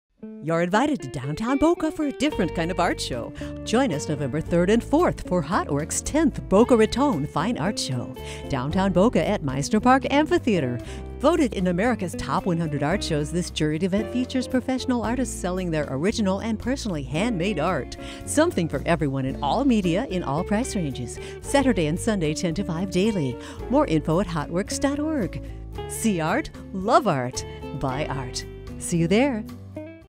Boca Raton Fine Art Show - November 3 & 4, 2018 - Mizner Park Amphitheater
Hotworks.Org presents to Boca Raton Fine Art show. Located in beautiful Mizner Park in downtown Boca Raton, The Boca Raton Fine Art Show will showcase of up 150 renowned Juried Artists. Come join us and see for yourself why the Boca Raton Fine Art Show is voted as one of the Top 100 art shows in America!